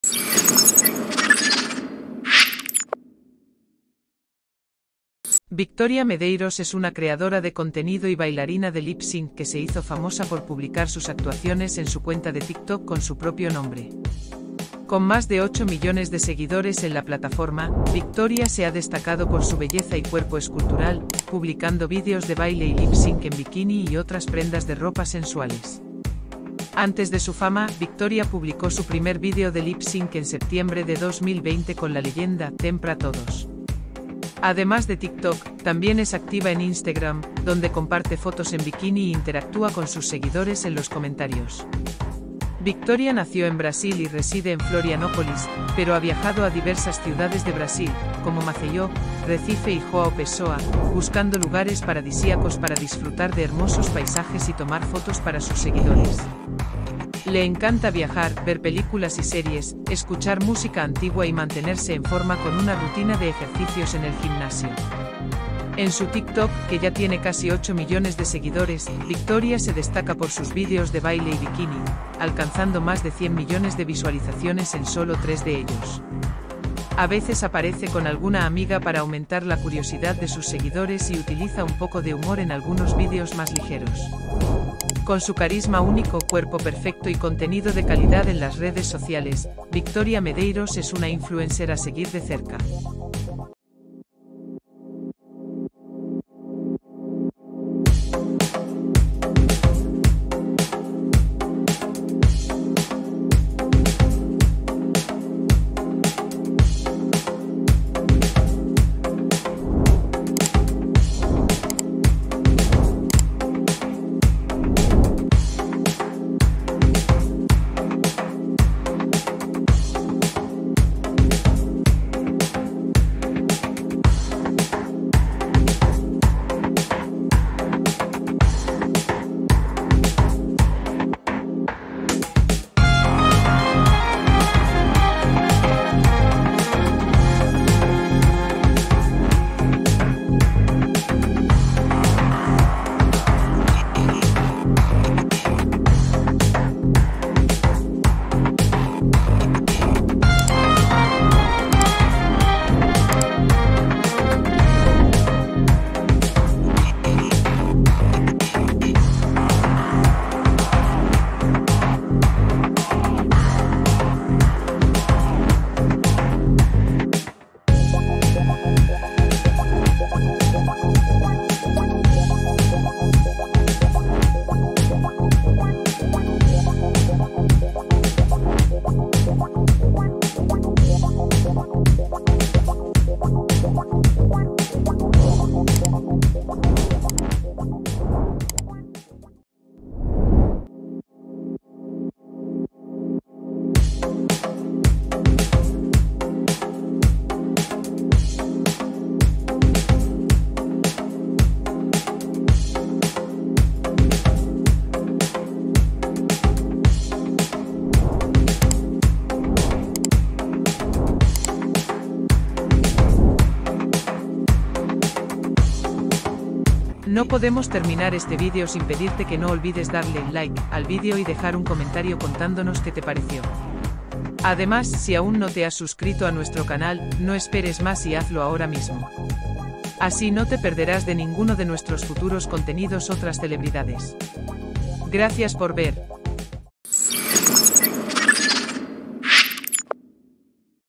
Victoria Medeiros: la belleza brasileña que conquistó el mundo con su baile y contenido en bikini
Victoria Medeiros es una creadora de contenido y bailarina de lip sync que se hizo famosa por publicar sus actuaciones en su cuenta de TikTok con su propio nombre. Con más de 8 millones de seguidores en la plataforma, Victoria se ha destacado por su belleza y cuerpo escultural, publicando vídeos de baile y lip sync en bikini y otras prendas de ropa sensuales.
Antes de su fama, Victoria publicó su primer video de lip sync en septiembre de 2020 con la leyenda "Tem pra todos". Además de TikTok, también es activa en Instagram, donde comparte fotos en bikini y interactúa con sus seguidores en los comentarios.
Victoria nació en Brasil y reside en Florianópolis, pero ha viajado a diversas ciudades de Brasil, como Maceió, Recife y João Pessoa, buscando lugares paradisíacos para disfrutar de hermosos paisajes y tomar fotos para sus seguidores. Le encanta viajar, ver películas y series, escuchar música antigua y mantenerse en forma con una rutina de ejercicios en el gimnasio.
En su TikTok, que ya tiene casi 8 millones de seguidores, Victoria se destaca por sus videos de baile y bikini, alcanzando más de 100 millones de visualizaciones en solo tres de ellos. A veces aparece con alguna amiga para aumentar la curiosidad de sus seguidores y utiliza un poco de humor en algunos videos más ligeros.